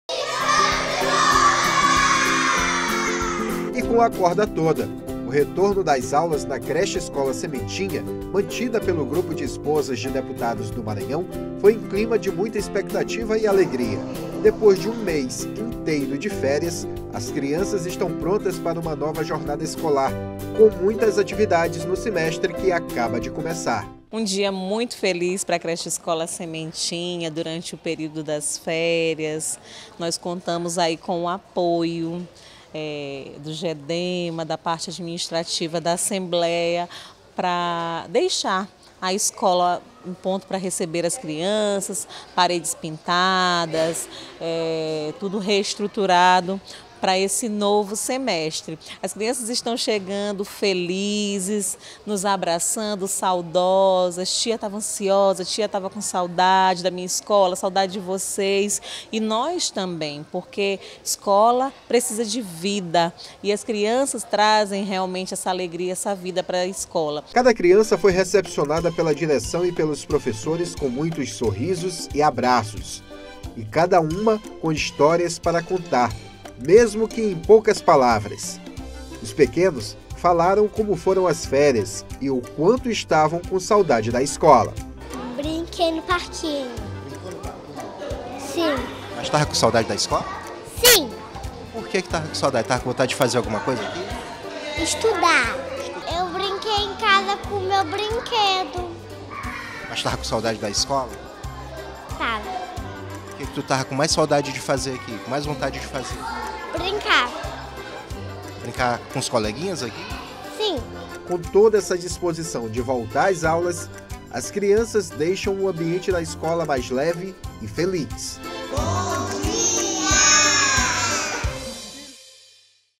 Educação: confira como foi o retorno às aulas dos alunos da Creche Escola Sementinha
A Creche-escola Sementinha voltou as atividades neste segundo semestre. a instituição de ensino é mantida pelo Grupo de Esposas de Deputados (Gedema).